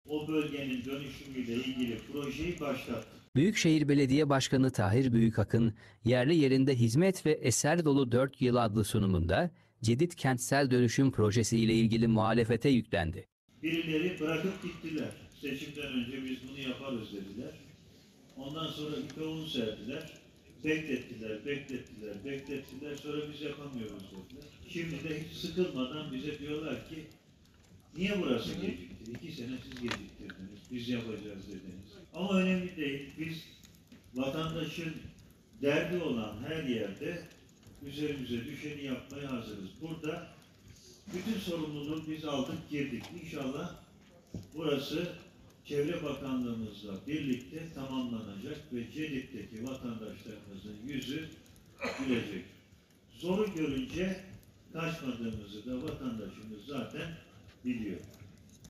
MUHALEFETE CEDİT GÖNDERMESİ
Büyükşehir Belediye Başkanı Tahir Büyükakın, Yerli Yerinde Hizmet ve Eser Dolu 4 yıl adlı sunumunda Cedit Kentsel Dönüşü Projesi ile ilgili muhalefete yüklendi